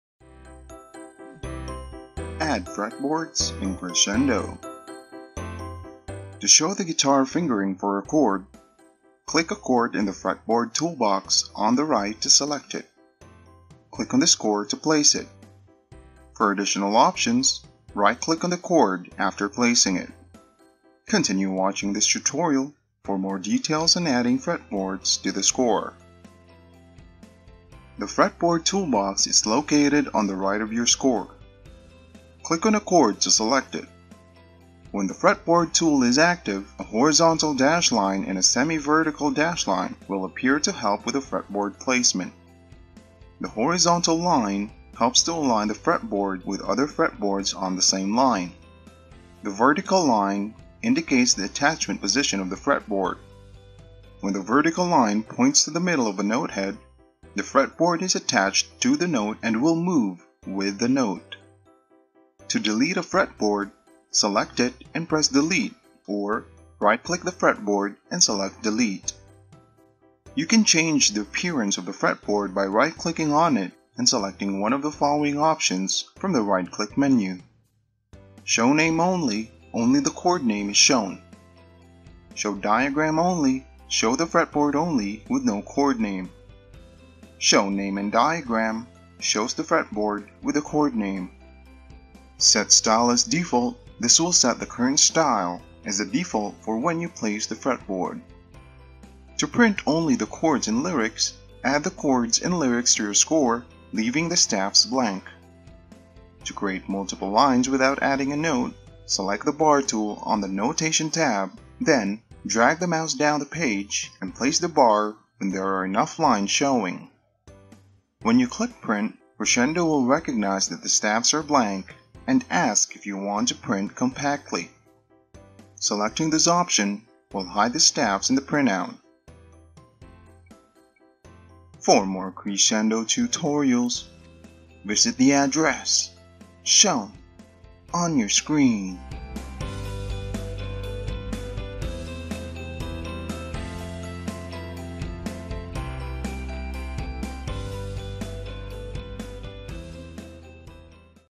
Crescendo 음악 악보 사보 프로그램 - 프렛보드(지판) 추가하기
이 비디오에서는 프렛보드(지판)을 악보 사보 작업에 어떻게 추가하는지를 설명해 드립니다. 프렛보드를 만들어 정렬하는 방법, 또 삭제하는 방법들도 재현해 보여드립니다. 단일 음표에 프렛보드를 넣는 방법, 오선 악보를 제외하고 코드와 가사만을 프린트하는 방법을 배우실 수 있습니다.

더 많은 Crescendo 악보 사보 편집기 튜토리얼을 보려면 , 여기를 방문하세요

Crescendo 사용법에 관해 도움이 필요하세요?